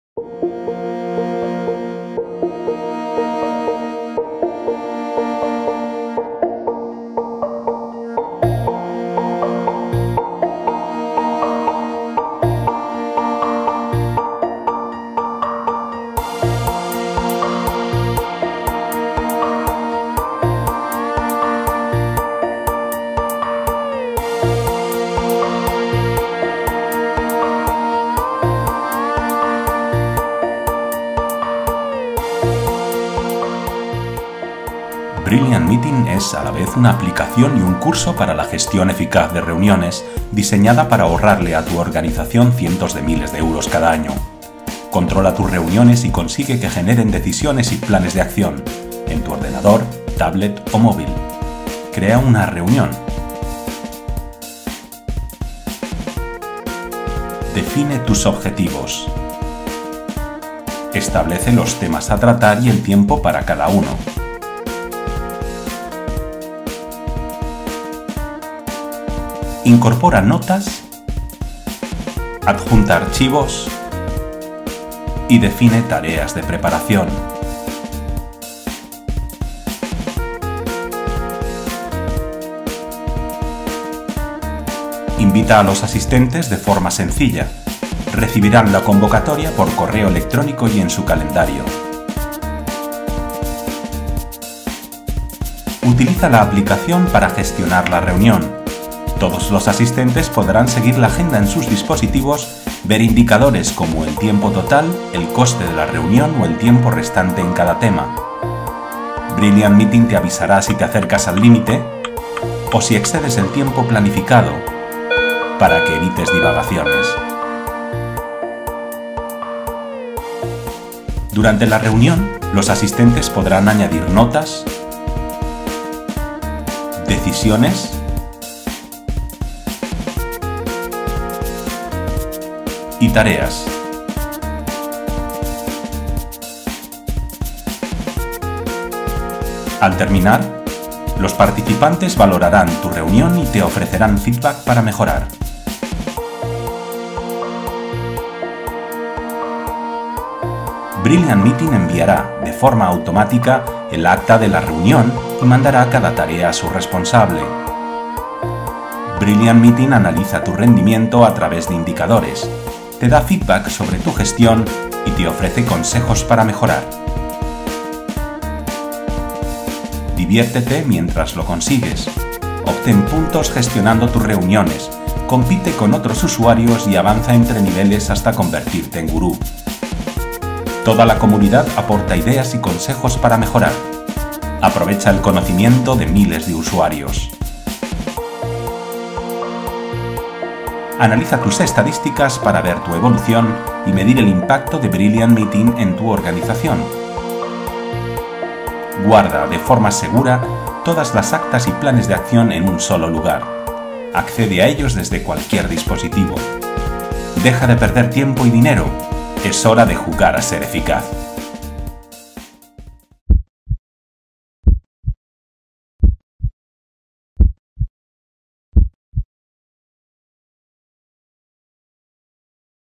Videodemo Brilliant Meeting (Español)
Brilliant Meeting es una aplicación-curso gamificada que hará que tus reuniones generen decisiones y planes de acción.
El empleado medio desperdicia 31 horas cada mes en reuniones ineficientes. El coste global de estas reuniones supera los 800.000 millones de euros anuales. Deja de perder tiempo y dinero. Es hora de jugar a ser eficaz